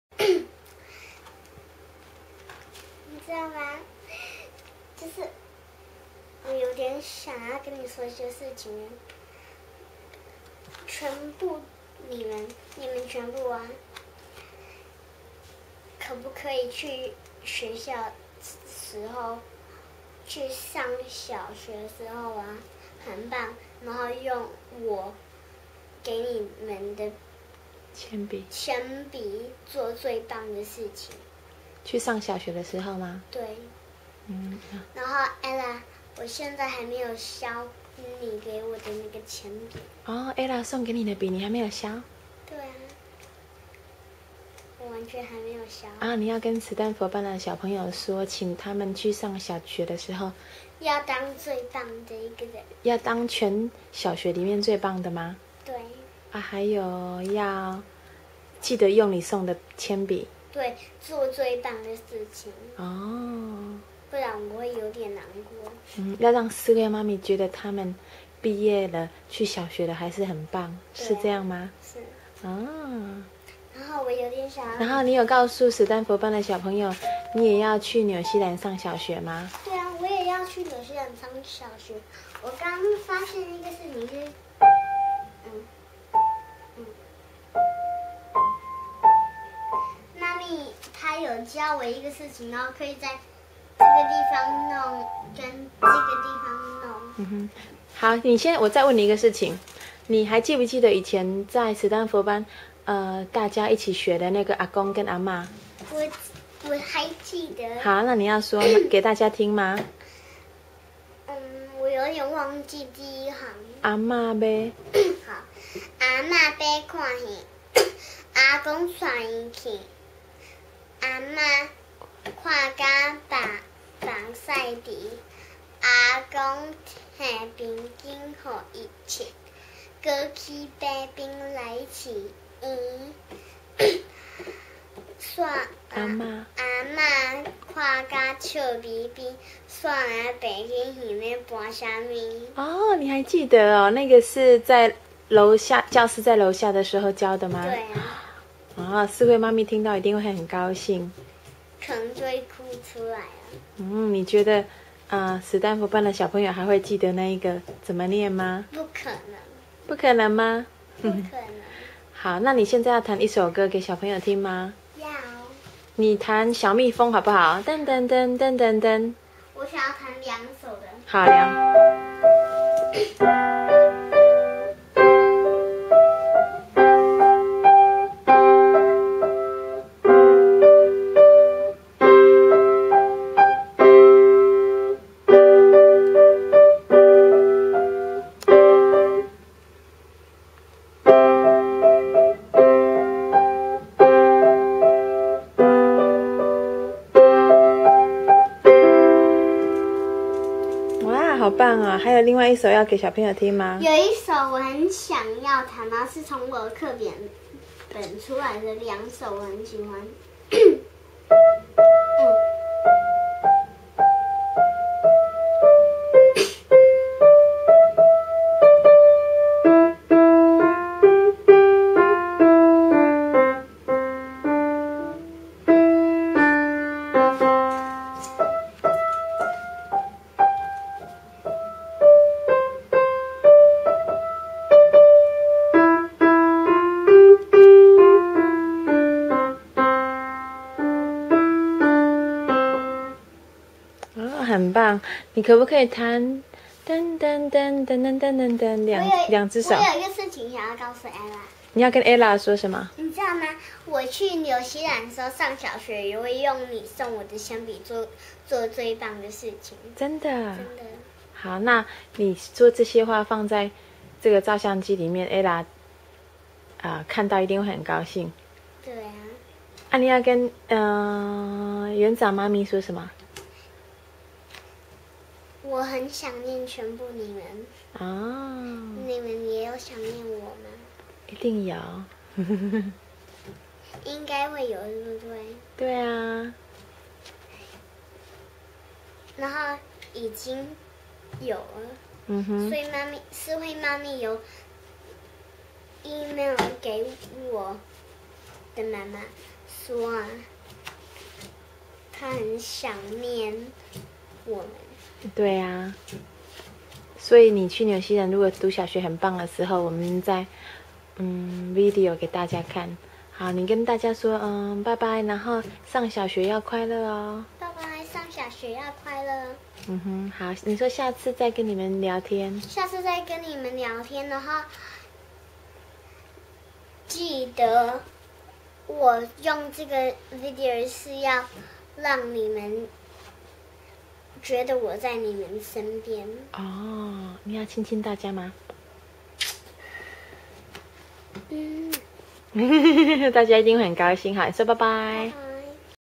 SKY的畢業感言.MP4
SKY的畢業感言給想念的史丹佛班的朋友們還有思慧+園長媽咪.